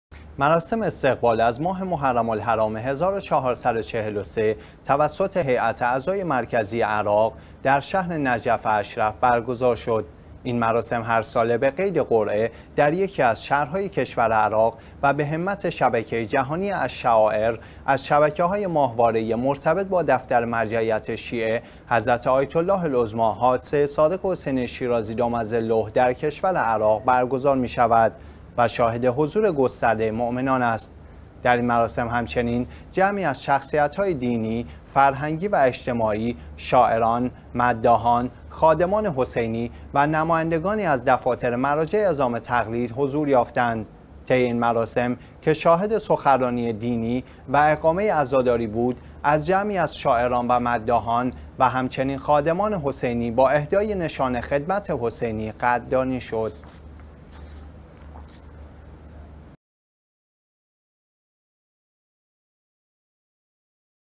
برگزاری مراسم استقبال از ماه محرم توسط هیئت عزای مرکزی عراق در شهر مقدس نجف
مراسم استقبال از ماه محرم الحرام 1443 توسط هیئت عزای مرکزی عراق در شهر نجف اشرف برگزار شد.
این مراسم هر ساله به قید قرعه در یکی از شهرهای کشور عراق و به همت شبکه جهانی الشعائر، از شبکه های ماهواره ای مرتبط با دفتر مرجعیت شیعه، حضرت آیت الله العظمی حاج سید "صادق حسینی شیرازی" (دام ظله)، در کشور عراق برگزار می شود و شاهد حضور گسترده مؤمنان است.
در این مراسم همچنین جمعی از شخصیت های دینی، فرهنگی و اجتماعی، شاعران، مداحان، خادمان حسینی و نمایندگانی از دفاتر مراجع عظام تقلید حضور یافتند.
طی این مراسم که شاهد سخنرانی دینی و اقامه عزاداری بود، از جمعی از شاعران و مداحان و همچنین خادمان حسینی با اهدای نشان خدمت حسینی قدردانی شد.